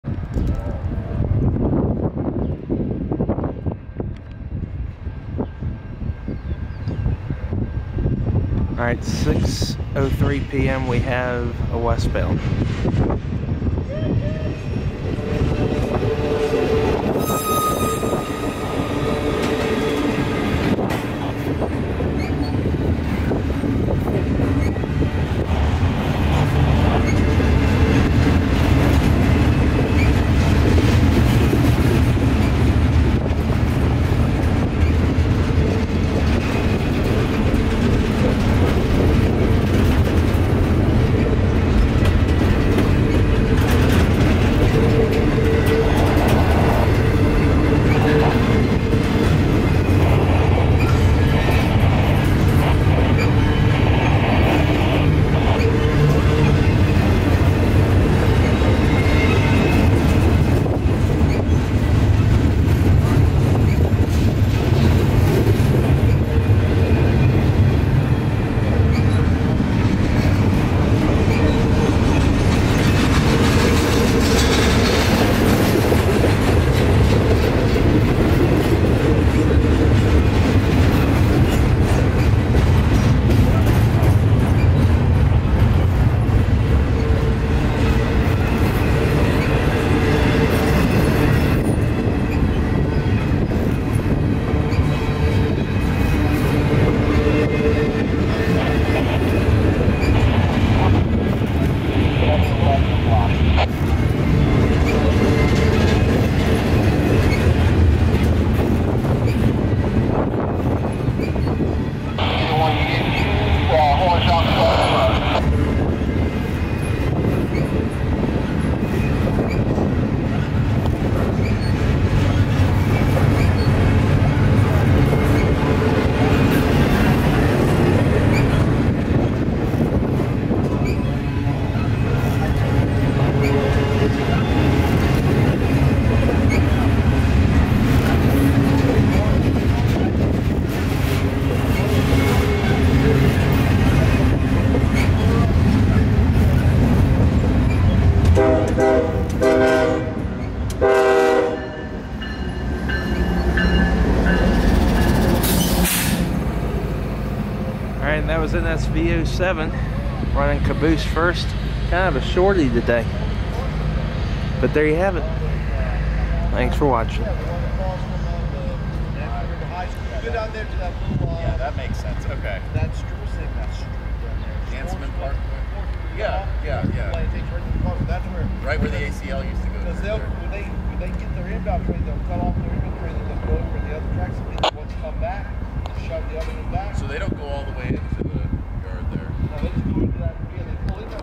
NS V07
6:03 PM

An earlier than usual NS V07 reverses into downtown Suffolk with a shorter train than he typically is. The engineer gave us all a nice horn salute too! Shot in 4K with a Sony Alpha a6400.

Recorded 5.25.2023